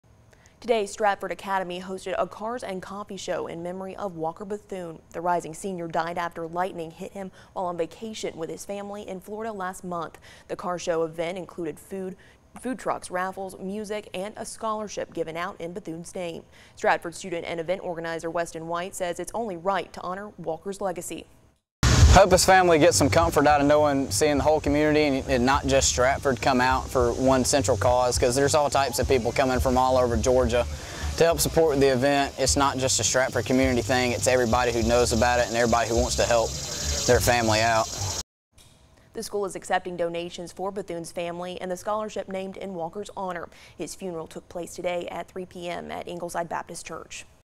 Stratford Academy holds 'Cars and Coffee' show in honor of Walker Bethune
Today, Stratford academy hosted a cars and coffee show in memory of Walker Bethune. The rising senior died after lightening hit him while on vacation with his family in Florida last month. The car show event included food trucks, raffles, music, and a scholarship given out in Bethune's name.

13WMAZ News is the #1 station for news and weather in the Central Georgia area, serving people in Dublin, Macon, Milledgeville and Warner Robins.